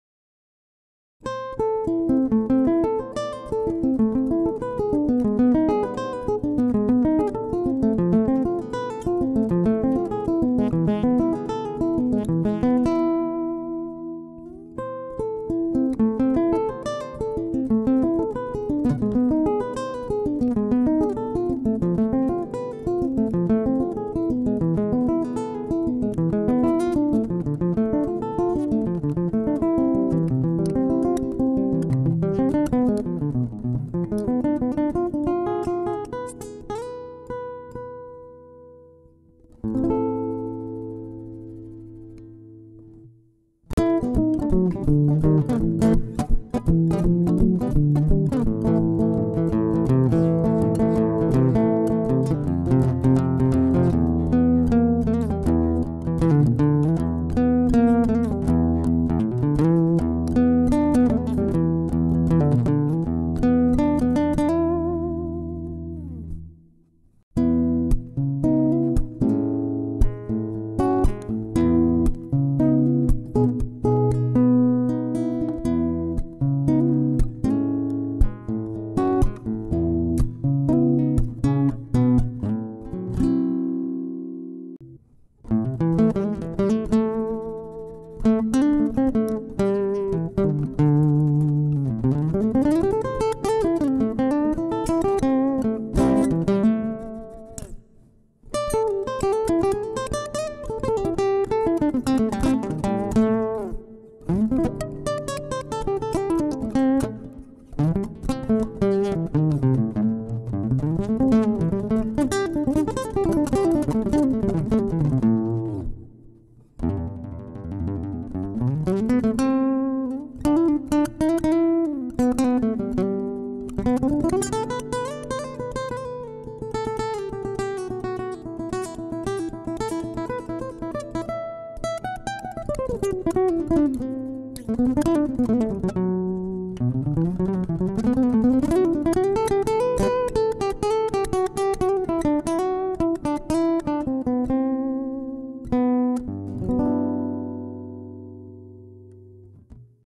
Trying Out My New Ibanez AEG8TNE-NT Semi-acoustic Guitar
Trying out my new Ibanez, loving the way it feels and sounds so far!